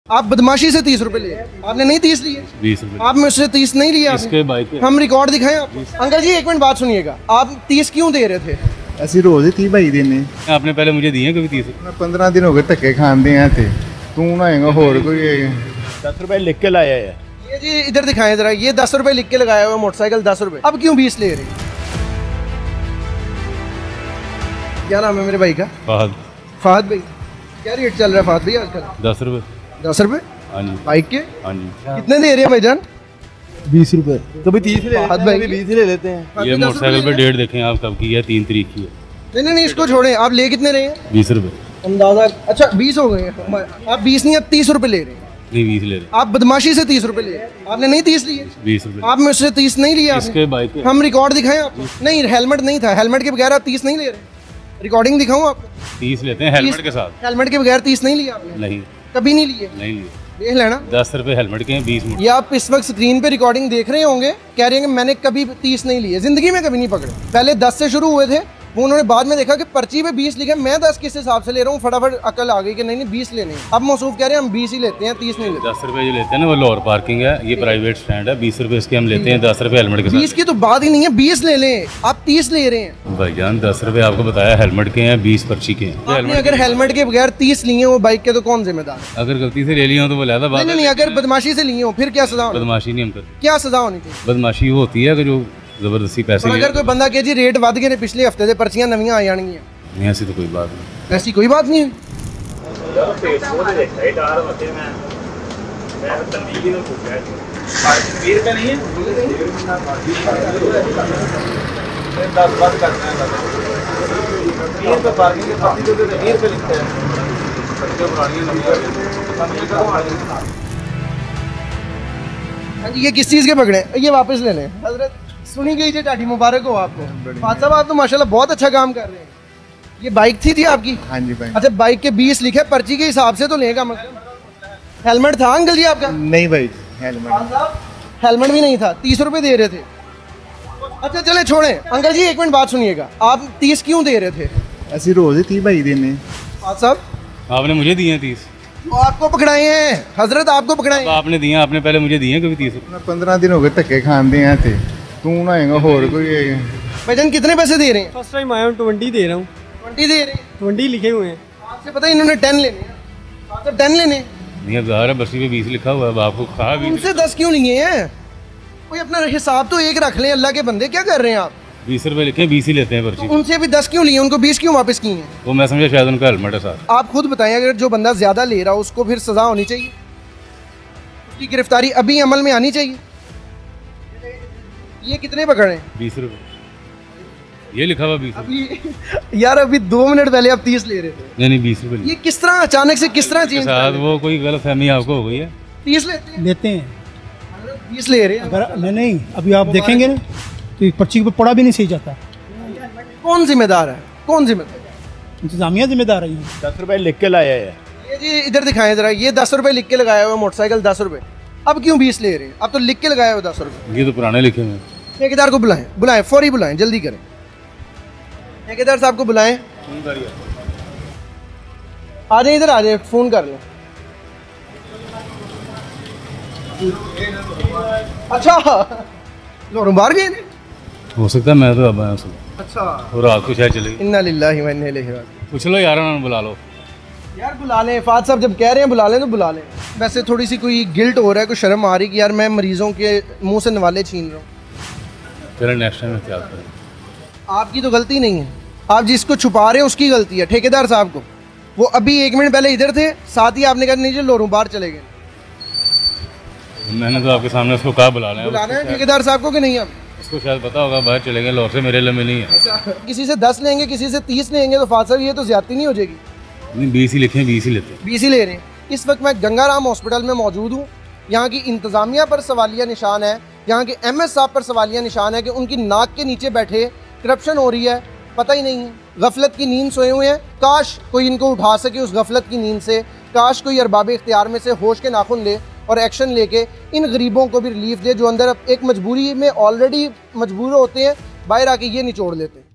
Operation Against Parking Mafia | SA Times
پارکنگ مافیا کی کھلے عام لوٹ مار
پاکستانیوں!  ان کا کیا علاج ہے؟